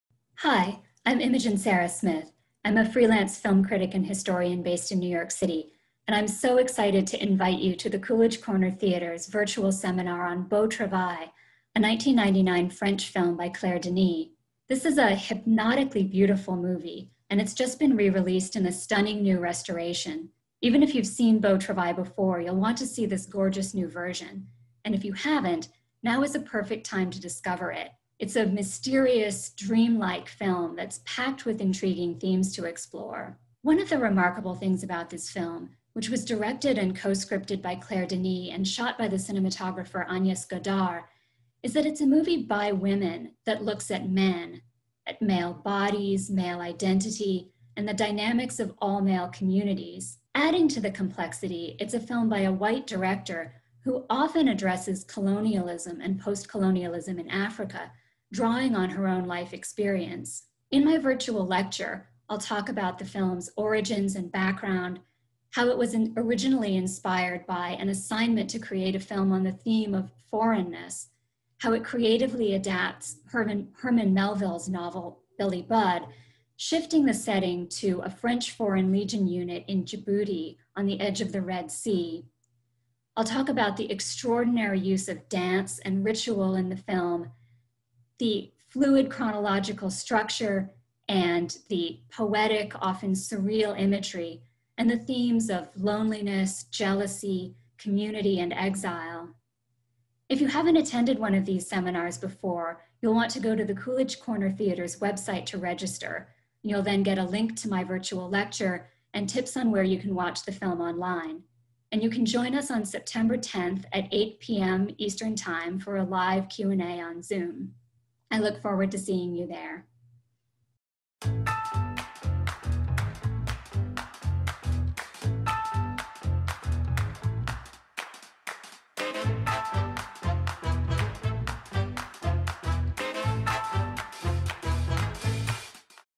Seminar: Beau Travail | Promo | Coolidge Corner Theatre
Film critic and historian Imogen Sara Smith leads this virtual seminar on Claire Denis' hypnotically beautiful drama.